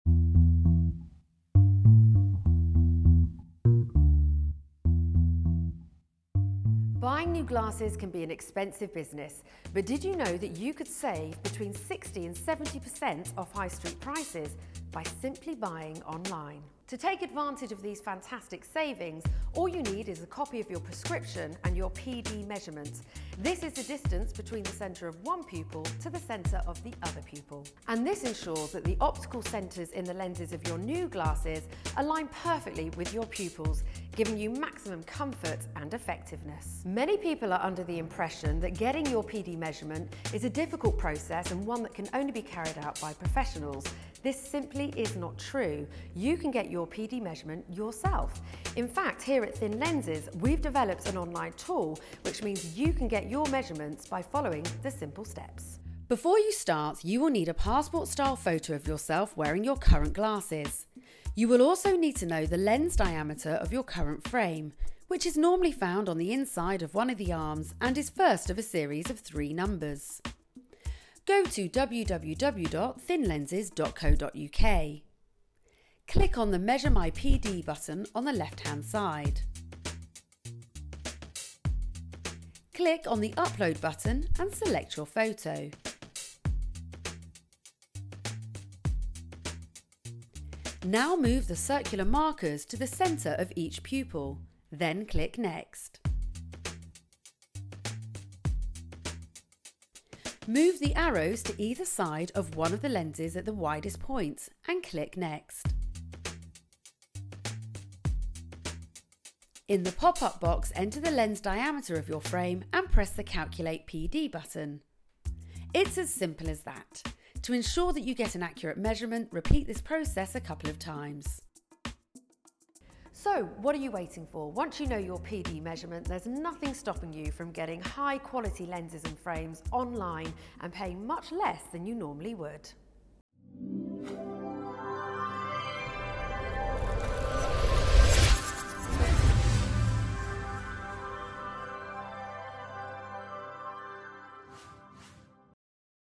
Measure your PD (Pupillary Distance) quickly and accurately
At ThinLenses, we have developed a tool that allows you to measure your PD online and get an accurate PD measurement within seconds.